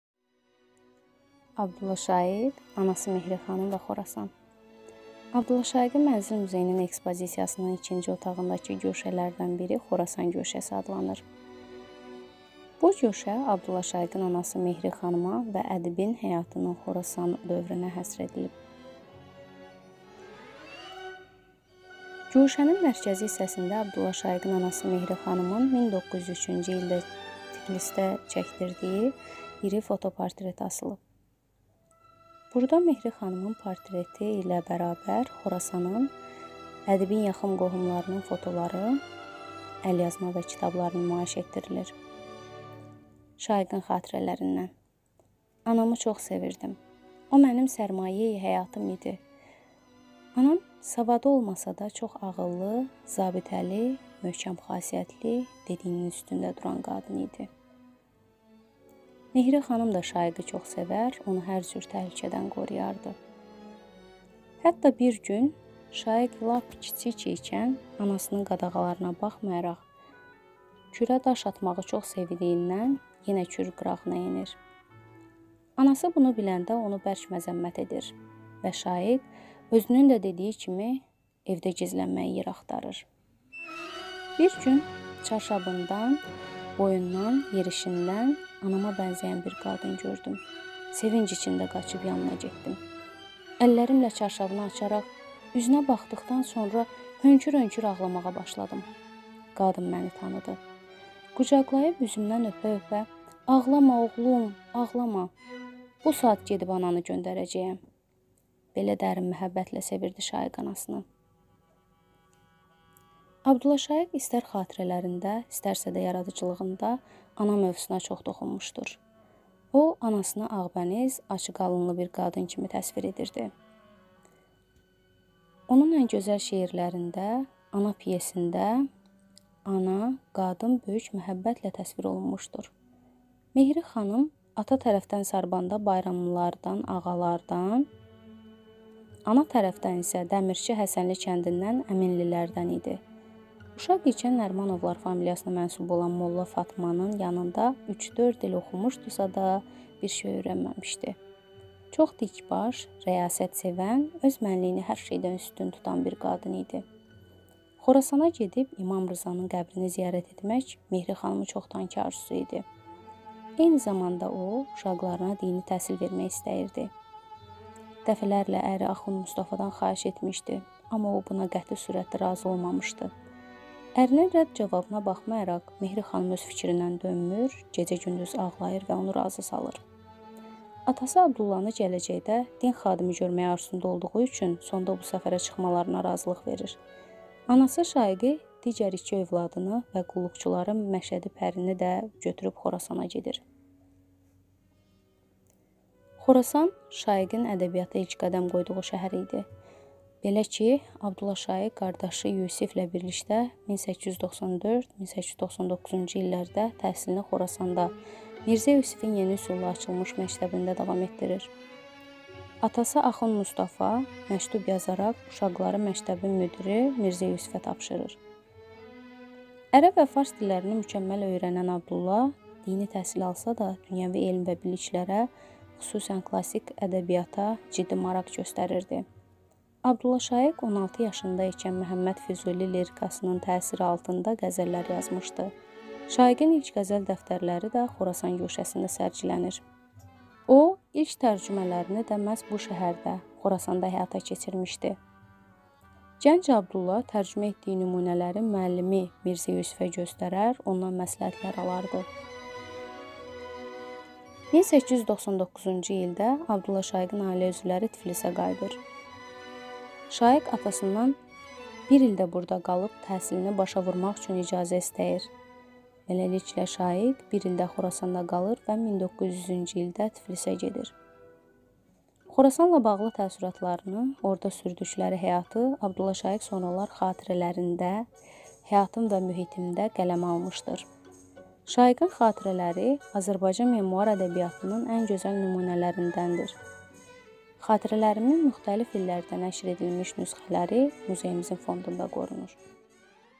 Abdulla Şaiqin Mənzil-Muzeyi. Xorasan guşəsi
Abdulla Şaiq, anası Mehri xanım və Xorasan...
Abdulla Şaiqin Mənzil-Muzeyinin ekspozisiyasının ikinci otağındakı guşələrdən biri Xorasan guşəsi adlanır. Bu guşə Abdulla Şaiqin anası Mehri xanıma və ədibin həyatının Xorasan dövrünə həsr edilib.  Guşənin mərkəzi hissəsində Abdulla Şaiqin anası Mehri xanımın 1903-cü ildə Tiflisdə çəkdirdiyi iri foto-portreti asılıb. Burada Mehri xanımın portreti ilə bərabər Xorasanın, ədibin yaxın qohumlarının fotoları, əlyazma və kitablar nümayiş etdirilir.
Şaiqin xatirələrindən...
Anamı çox sevirdim, o mənim sərmayeyi-həyatım idi... Anam savadı olmasa da, çox ağıllı, zabitəli, möhkəm xasiyyətli, dediyinin üstündə duran qadın idi…
Mehri xanım da Şaiqi çox sevər, onu hər cür təhlükədən qoruyardı. Hətta bir gün Şaiq lap kiçik ikən anasının qadağalarına baxmayaraq, Kürə daş atmağı çox sevdiyindən yenə Kür qırağına enir. Anası bunu biləndə onu bərk məzəmmət edir. Və Şaiq özünün də dediyi kimi "evdə gizlənməyə yer axtarır".
"Bir gün çarşabından, boyundan, yerişindən anama bənzəyən bir qadın gördüm. Sevinc içində qaçıb yanına getdim. Əllərimlə çarşabını açaraq, üzünə baxdıqdan sonra hönkür-hönkür ağlamağa başladım. Qadın məni tanıdı. Qucaqlayıb üzümdən öpə-öpə: "Ağlama oğlum, ağlama. Bu saat gedib ananı göndərəcəyəm". Belə dərin məhəbbətlə sevirdi Şaiq anasını. A.Şaiq istər xatirələrində, istərsə də yaradıcılığında ana mövzusuna çox toxunmuşdur. O, anasını ağbəniz, açıq alınlı bir qadın kimi təsvir edirdi. Onun ən gözəl şeirlərində, “Ana” pyesində ana, qadın böyük məhəbbətlə təsvir olunmuşdur.
Mehri xanım ata tərəfdən Sarbanda Bayramlılardan-ağalardan, ana tərəfdən isə Dəmirçi Həsənli kəndindən Əminlilərdən idi. Uşaq ikən Nərimanovlar familiyasına mənsub olan molla Fatmanın yanında üç-dörd il oxumuşdusa da bir şey öyrənməmişdi... Çox dikbaş, rəyasət sevən, öz mənliyini hər şeydən üstün tutan bir qadın idi... "
Xorasana gedib, İmam Rzanın qəbrini ziyarət etmək Mehri xanımın çoxdankı arzusu idi. Eyni zamanda o, uşaqlarına  dini təhsil vermək istəyirdi. Dəfələrlə əri Axund Mustafadan xahiş etmişdi, amma o buna qəti surətdə razı olmamışdı. Ərinin rədd cavabına baxmayaraq, Mehri xanım öz fikrindən dönmür, gecə-gündüz ağlayır və onu razı salır. Atası Abdullanı gələcəkdə din xadimi görmək arzusunda olduğu üçün sonda bu səfərə çıxmalarına razılıq verir. Anası Şaiqi, digər iki övladını və qulluqçuları Məşədi Pərini də götürüb Xorasana gedir.
Xorasan, Şaiqin ədəbiyyata ilk qədəm qoyduğu şəhər idi. Belə ki, A.Şaiq qardaşı Yusiflə birlikdə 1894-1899-cu illərdə təhsilini Xorasanda Mirzə Yusifin yeni üsulla açılmış məktəbində davam etdirir. Atası Axund Mustafa məktub yazaraq uşaqları məktəbin müdiri Mirzə Yusifə tapşırır. Ərəb və fars dillərini mükəmməl öyrənən Abdulla dini təhsil alsa da, dünyəvi elm və biliklərə, xüsusən klassik ədəbiyyata ciddi maraq göstərirdi. A.Şaiq 16 yaşında ikən Məhəmməd Füzuli lirikasının təsiri altında qəzəllər yazmışdı. Şaiqin ilk qəzəl dəftərləri də Xorasan guşəsində sərgilənir.
O, ilk tərcümələrini də məhz bu şəhərdə – Xorasanda həyata keçirmişdi. Gənc Abdulla tərcümə etdiyi nümunələri müəllimi Mirzə Yusifə göstərər, ondan məsləhətlər alardı.
1899-cu ildə A.Şaiqin ailə üzvləri Tiflisə qayıdır. Şaiq atasından bir il də burada qalıb, təhsilini başa vurmaq üçün icazə istəyir. Beləliklə, Şaiq bir il də Xorasanda qalır və 1900-cü ildə Tiflisə gedir.
Xorasanla bağlı təəssüratlarını, orada sürdükləri həyatı A.Şaiq sonralar “Xatirələr”ində, "Həyatım və mühitim"də qələmə almışdır. Şaiqin “Xatirələri” Azərbaycan memuar ədəbiyyatının ən gözəl nümunələrindəndir. “Xatirələrim”in müxtəlif illərdə nəşr edilmiş nüsxələri muzeyimizin fondunda qorunur.